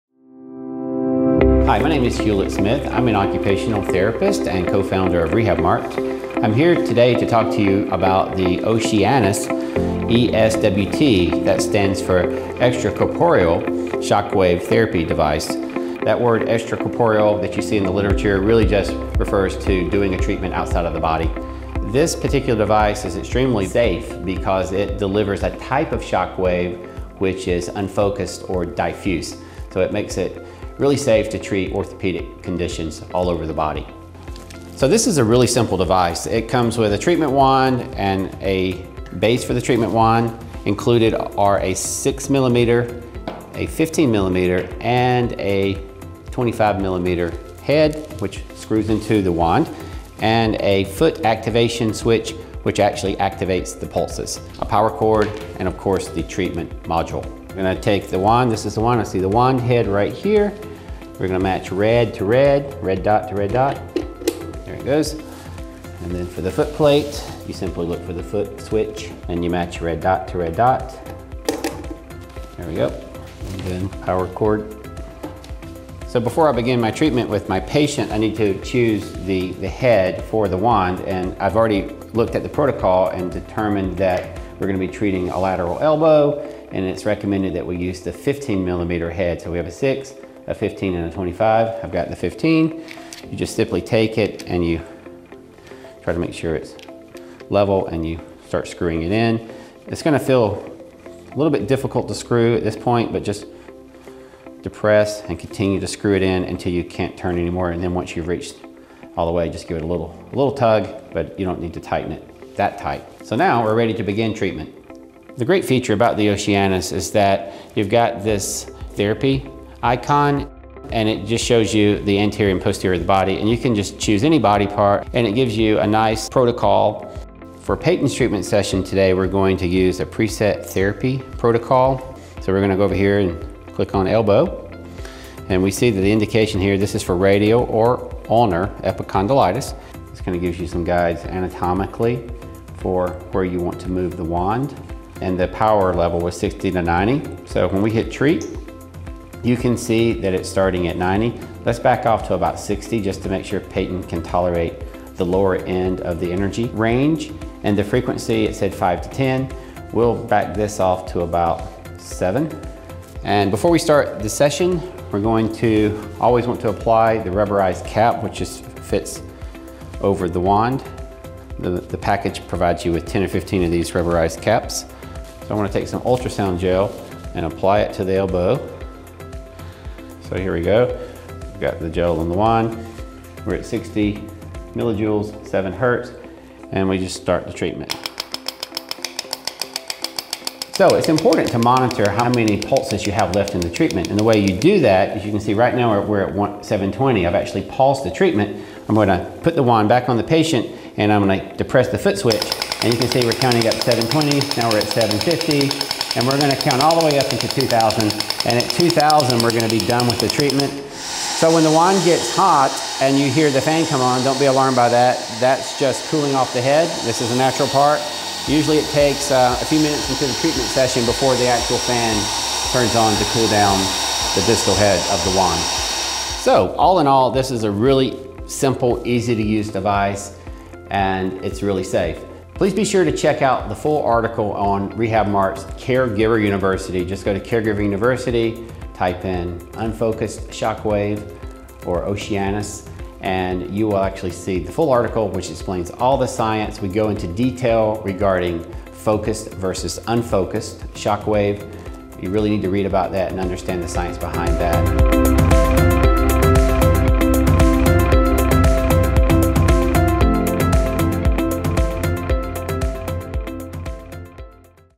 Oceanus Shockwave Therapy (SWT) Review & Demo
The Oceanus Shockwave Therapy (SWT) Device is a Class I FDA-registered device that delivers diffuse shockwave therapy with a cylindrical piston head. Specifically, it’s a low-energy ballistic device that uses an electromagnetic generator to deliver the acoustic waves to the affected area.

To learn more about shockwave therapy and the Oceanus device, check out our full article on Caregiver University